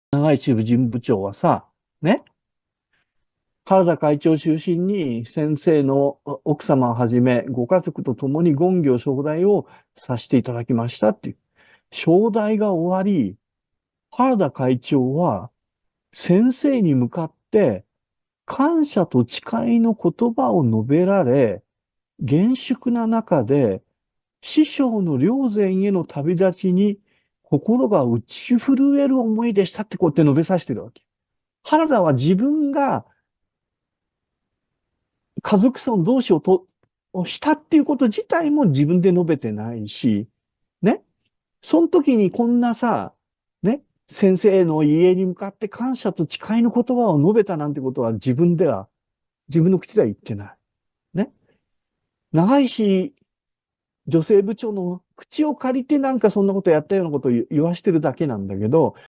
池田先生 家族葬 原田会長の言葉
本編(元動画)は動画画面内の 作成元 をタップしてご視聴下さい。

池田家vs原田執行部の対立
第38回教学研究会より(2024.2.3)